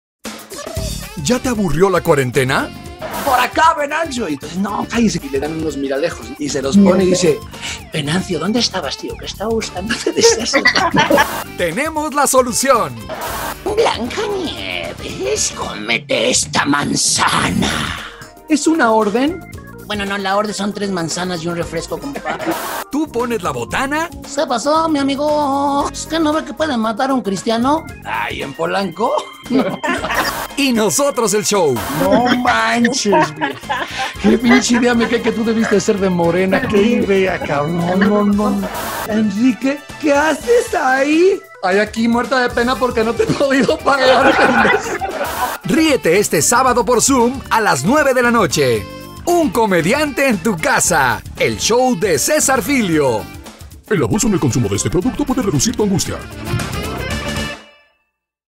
UnComedianteEnTuCasa
Únete a UnComedianteEnTuCasa el próximo sábado 4 de julio, y todos los siguientes, a las 9pm (hora centro) por ZOOM. Reune tu grupo de amigos o familiares, preparen su trago, botana, y pasen una noche diferente y llena de risas con Cesar Filio. COVER CERO CONTAGIO 200$ (por dispositivo) ¡Mándanos un mensaje o un WhatsApp al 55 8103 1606 y no te quedes fuera!